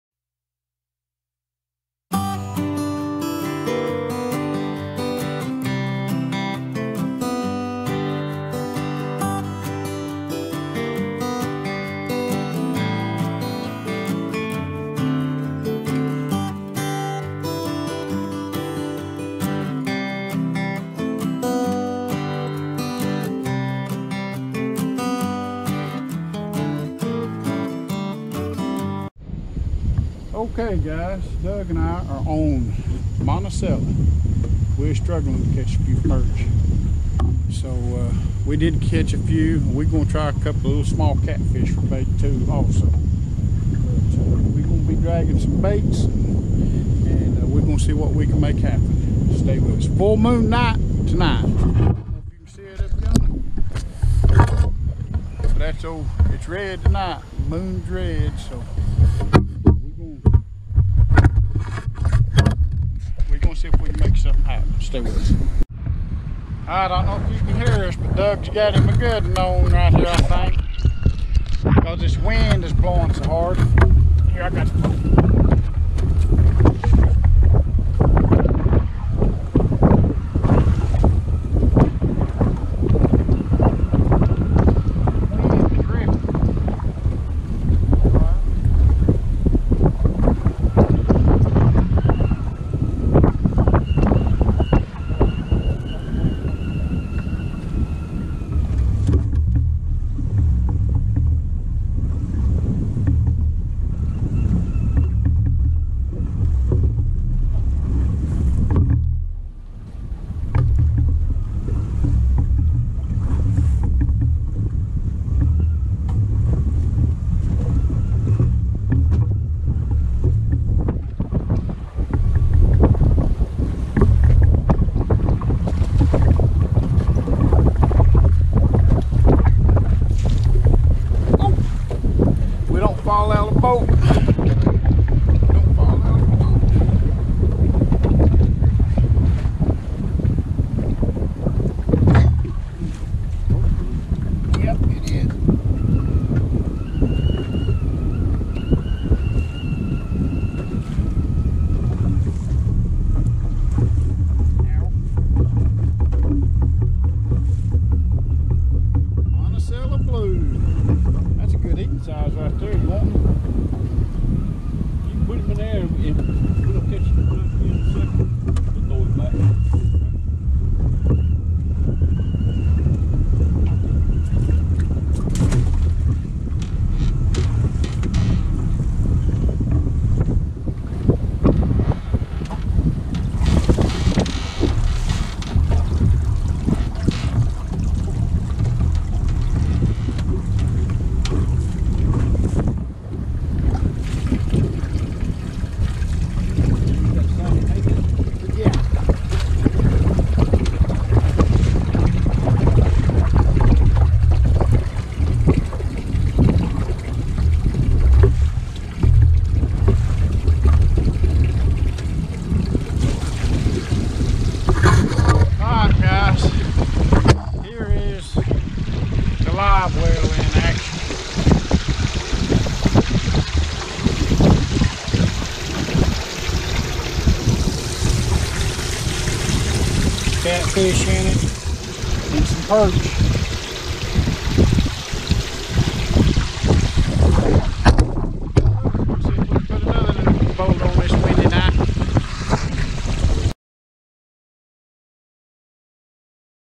Night Fishing in High Winds
Tough Fishing with full moon
and high winds